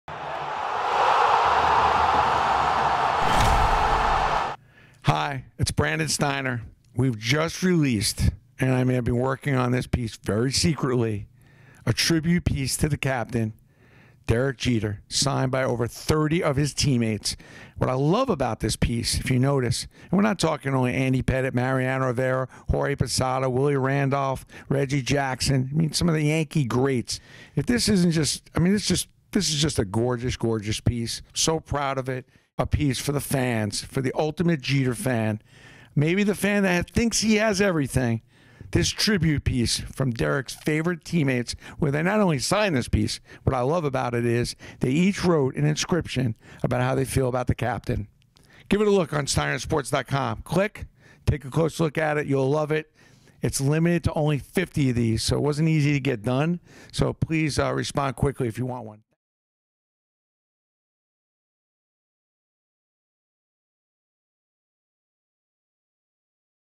The Derek Jeter Tribute Piece
This incredible 20x24 photo is a 6 month project that we have been working on at Steiner Sports. This is all about the story behind the numbers of retiring New York Yankees legend Derek Jeter.

This is signed by 34 current or former Yankees whom each wrote a few words about Jeter. The result: one of the greatest pieces we have ever created and a true "Tribute" to the Yankee Captain.